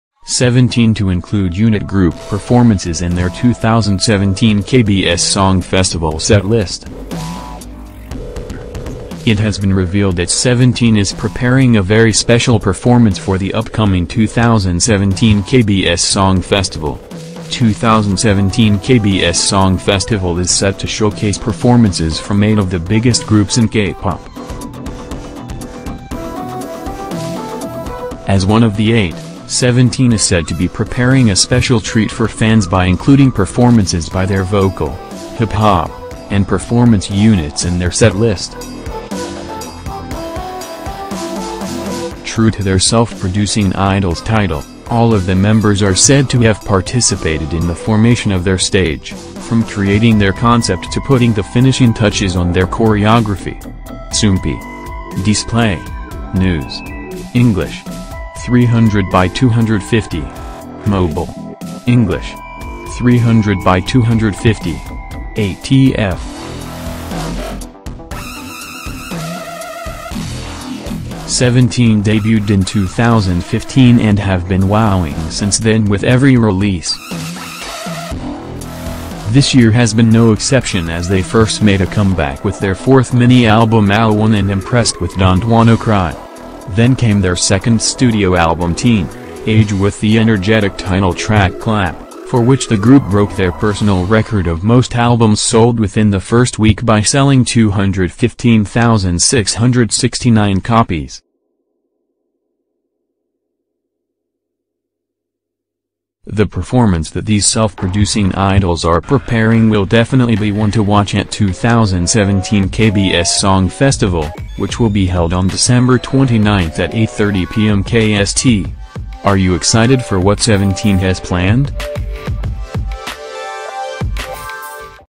SEVENTEEN To Include Unit Group Performances In Their 2017 KBS Song Festival Set List(News)
It has been revealed that SEVENTEEN is preparing a very special performance for the upcoming 2017 KBS Song Festival! 2017 KBS Song Festival is set to showc